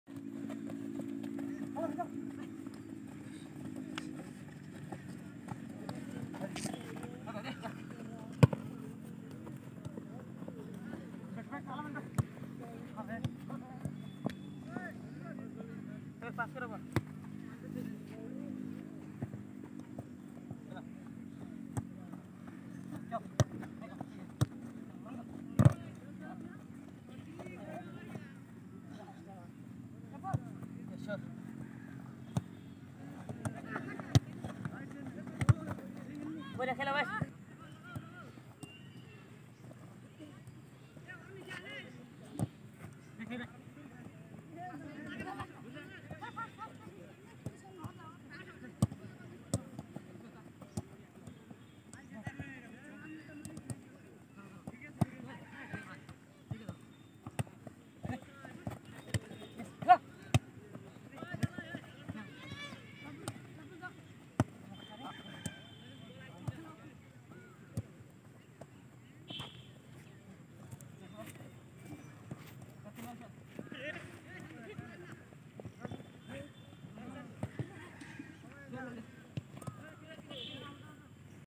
Rhmskill: 2V2 passing drill 2020
Rhmskill:
8 players
6 marker
2 mini goal post
4 bib